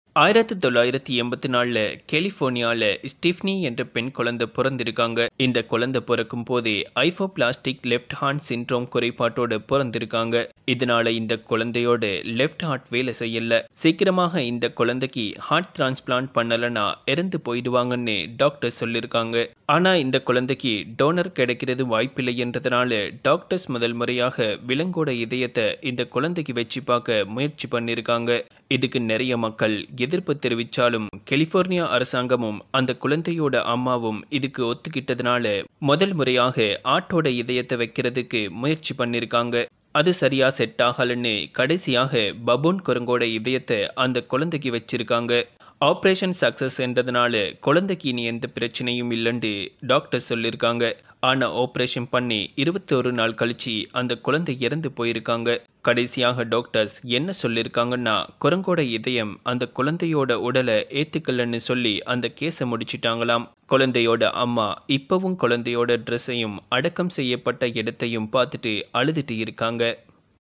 குரங்கோட இதயத்த மனிதனுக்கு பொருத்தினால் என்ன ஆகும்? | Real Story
ஒரு சிறு குழந்தை ஒன்டுக்கு குரங்கோட இதயத்த பொறுத்துருக்காங்க அந்த கொழந்த operation பண்ணி 21 நாளுக்கு அப்புறம் குரங்கோட இதயம் set ஆகலன்னு இறந்து போயிருக்காங்க.